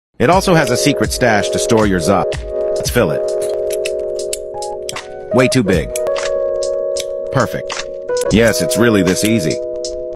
It Also Has a Secret Stash To Store Your Za! 💨
Best sunglasses designed for summer 2023! We have created the best stash to hide your pot. Just pull the pin and save it 😎 Imagine wearing these sunglasses to festivals!